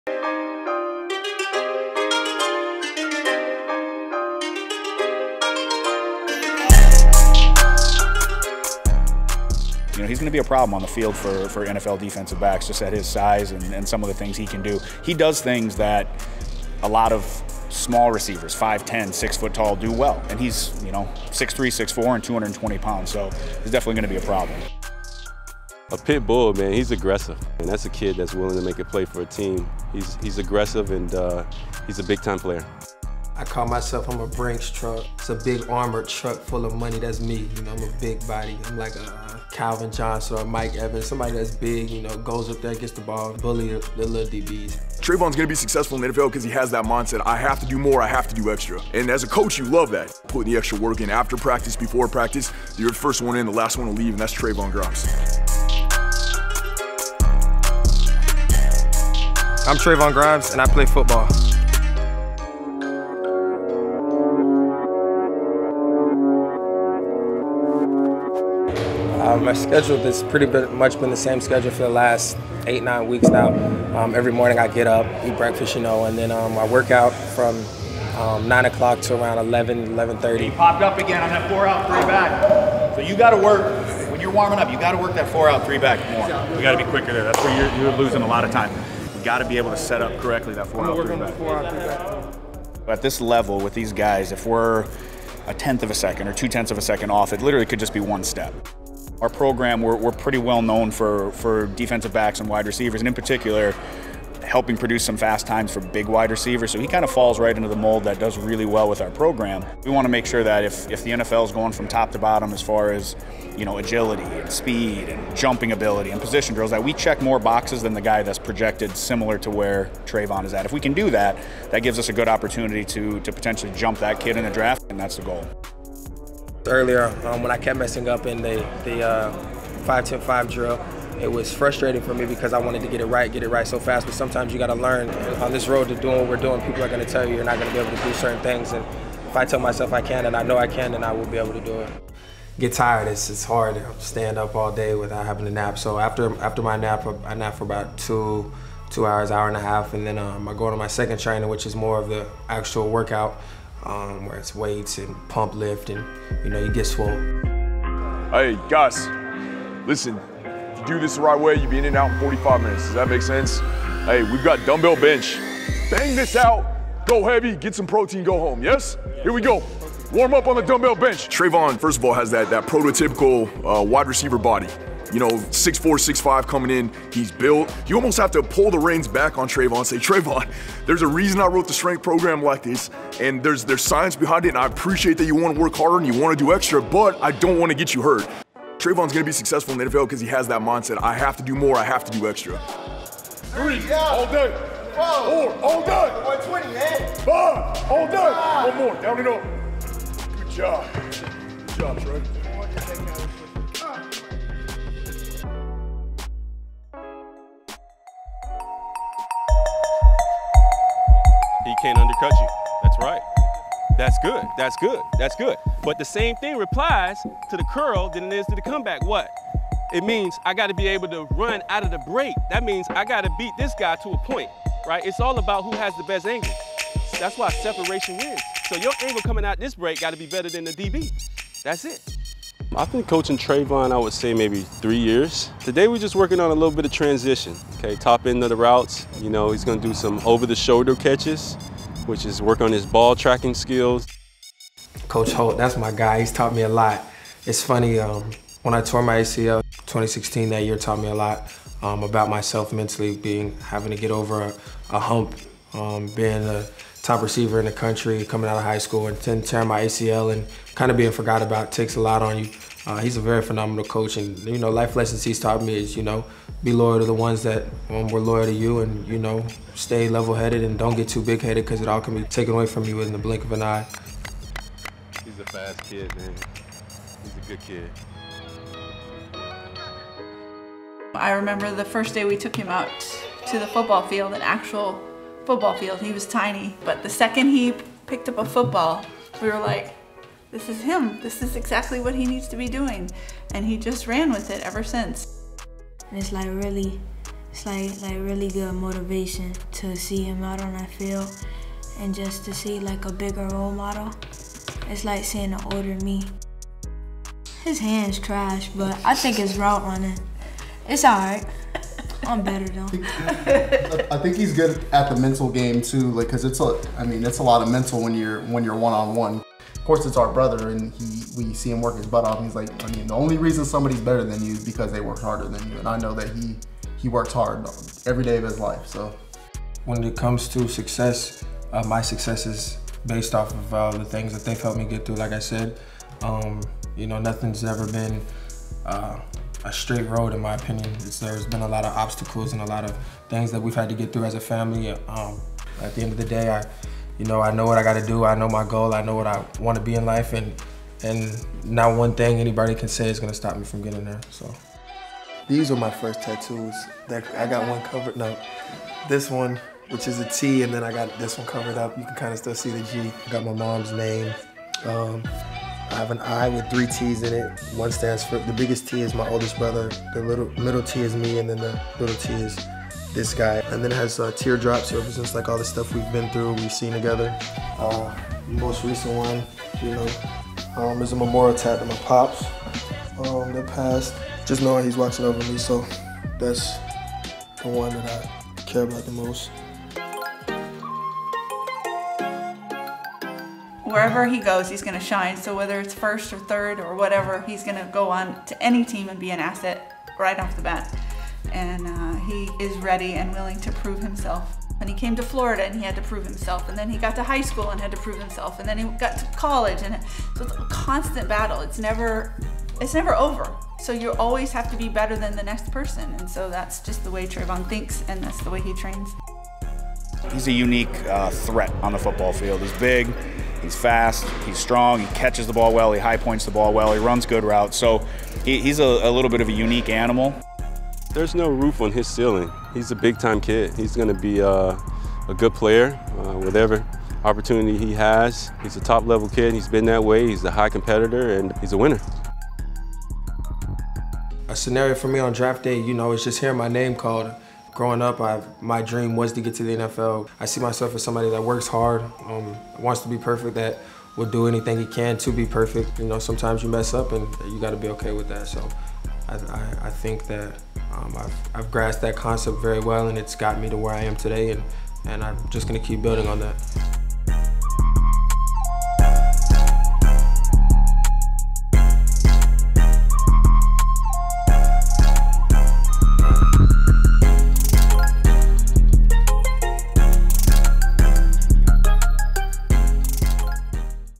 Philadelphia Eagles WR Trevon Grimes Wants To Be The NEXT Mike Evans! 👀
Florida Gators wide receiver Trevon Grimes has the size to DOMINATE at the next level!

Want a chance to be featured on your own episode of No Days Off? Apply here!

COMMENT with someone or something you want to see on the next episode of No Days Off!